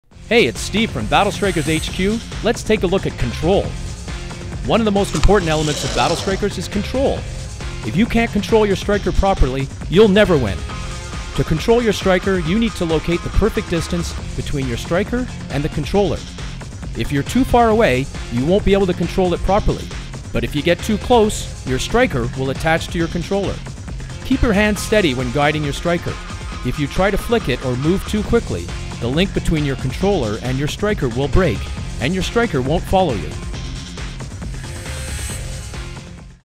Controlling Your Striker
Hey Battlers! Control is one of the most important skills you have to learn to overcome your opponents. This video will teach you how to control your Striker flawlessly!